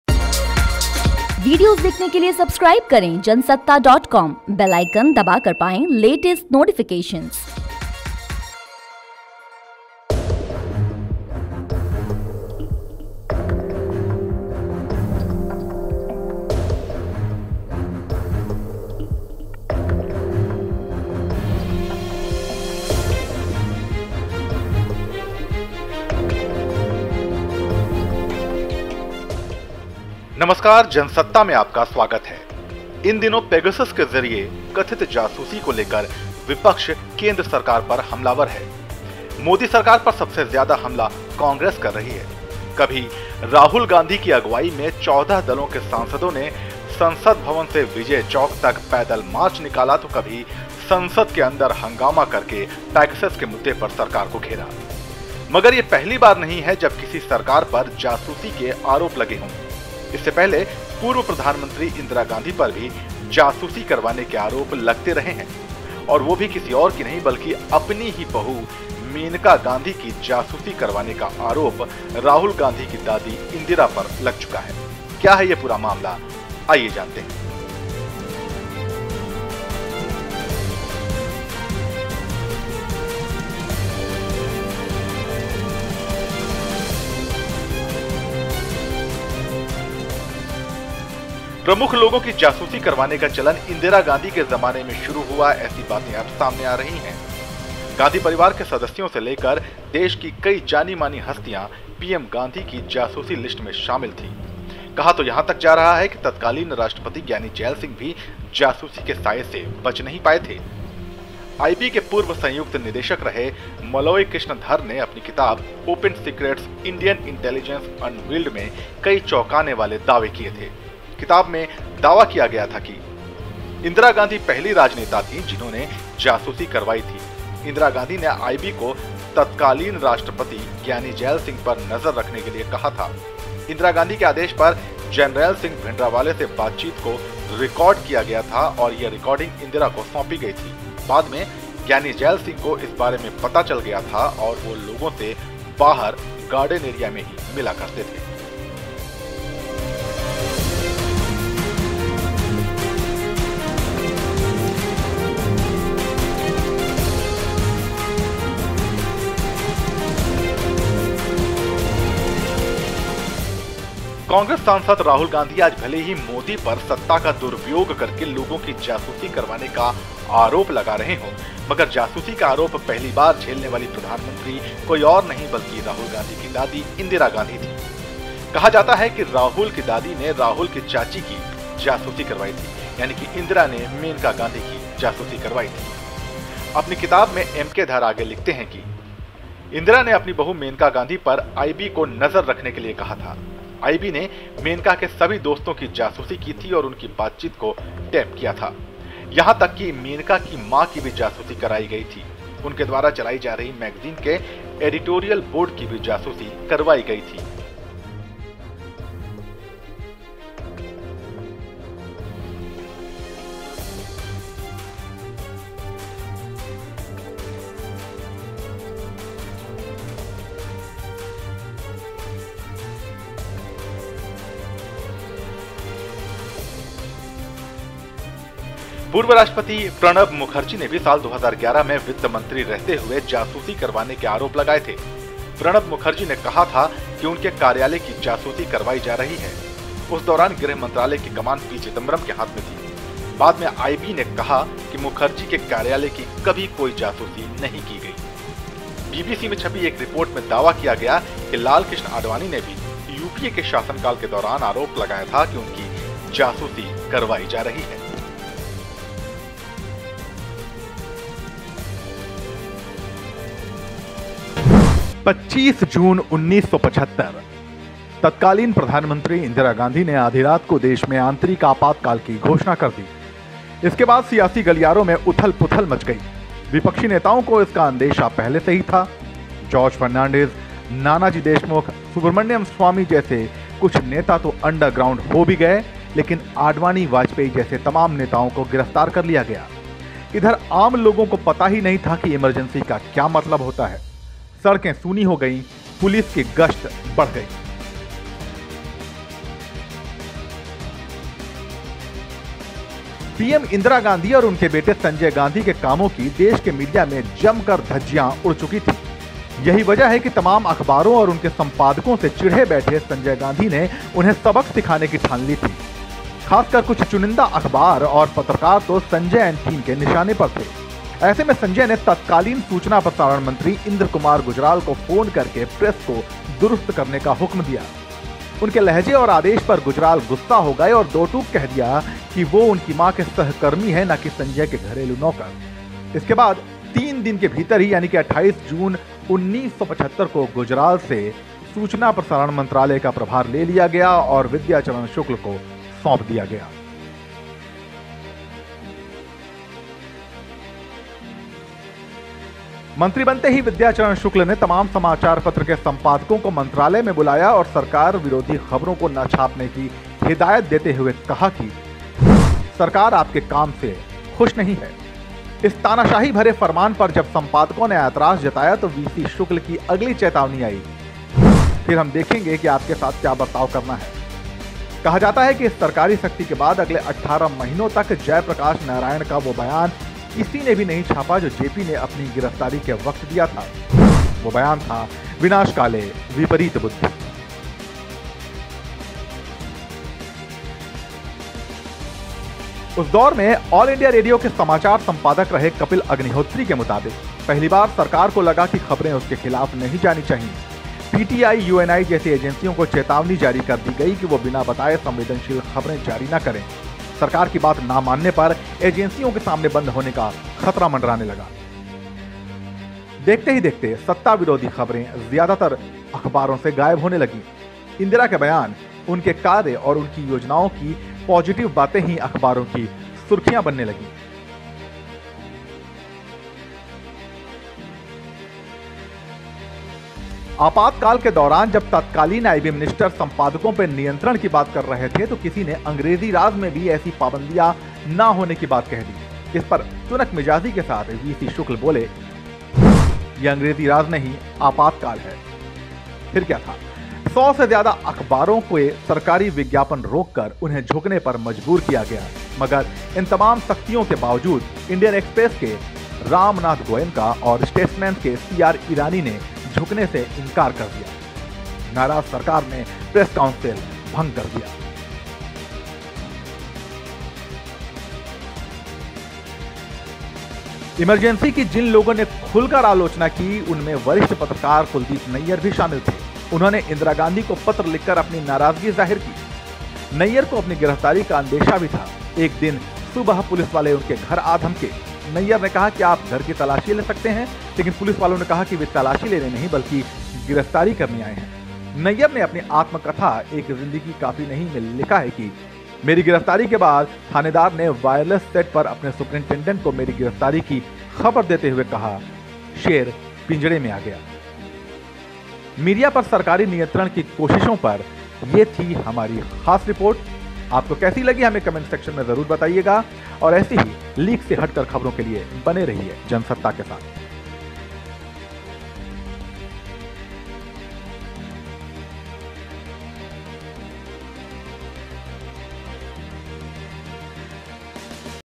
Pegasus से काफी पहले Indira Gandhi ने कराई थी बहू मेनका की जासूसी, Ex IB Officer की किताब में खुलासा
Indira Gandhi Spying Her Daughter in Law Menka Gandhi: पेगासस जासूसी कांड (Pegasus Spy-wear) को लेकर पीएम मोदी (PM Modi) और केन्द्र की बीजेपी सरकार (BJP Government) को लगातार घेरने वाली कांग्रेस (Congress) के लिए ये खबर थोड़ा चौंकाने वाली हो सकती है...आईबी (IB) के पूर्व अधिराकी एम.के. धर (M.K.Dhar) ने अपनी किताब में खुलासा किया है कि देश के अंदर खास लोगों की जासूसी का चलन राहुल गांधी (Rahul Gandhi) की दादी और पूर्व प्रधानमंत्री इंदिरा गांधी (Indira Gandhi) ने ही शुरु किया था...धर के मुताबिक इंदिरा ने तत्कालिन राष्ट्रपति (President of India) ज्ञानी जैल सिंह (Gyani Jail Singh) और अपनी बहू मेनका गांधी (Menka Gandhi) तक की जासूसी करवाई थी...पेश है जनसत्ता की ये खास रिपोर्ट...